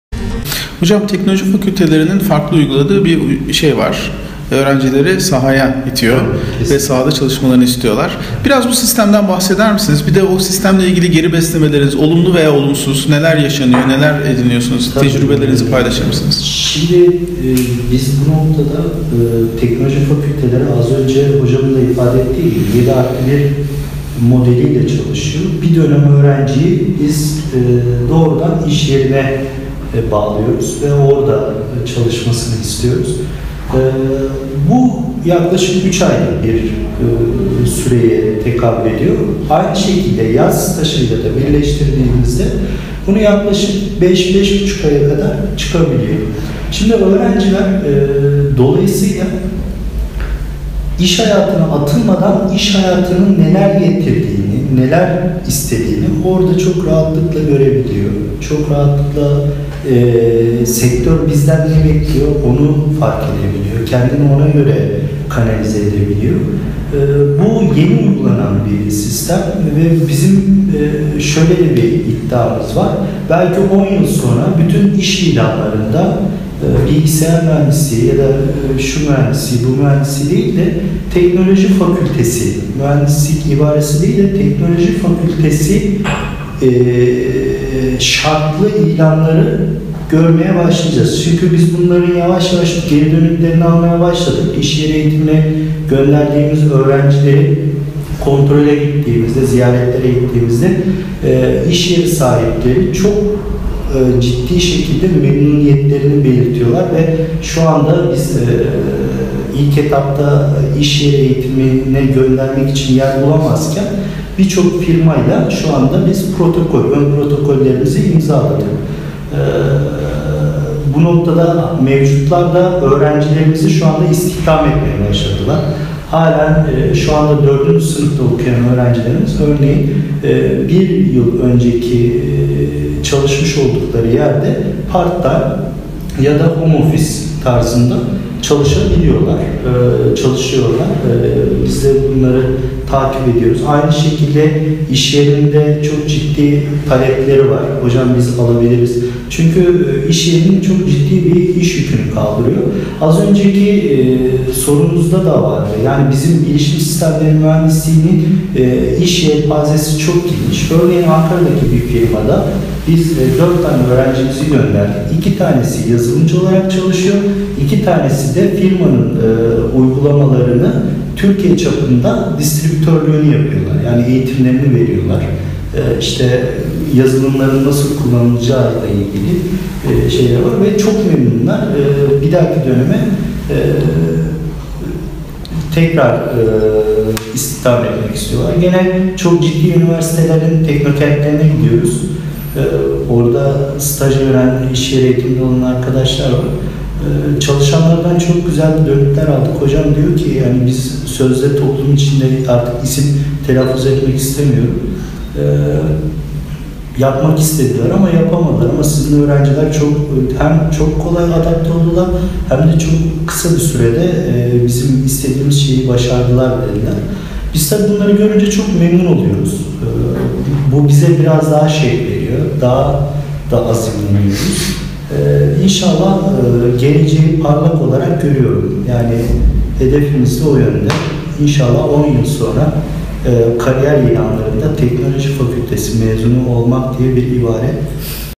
Teknoloji Fakülteleri ve Staj Farkı
Muğla Sıtkı Koçman Üniversitesi Bilişim Sistemleri Mühendisliği Bölümü Öğretim Üyesi Hocalarımıza soruyoruz.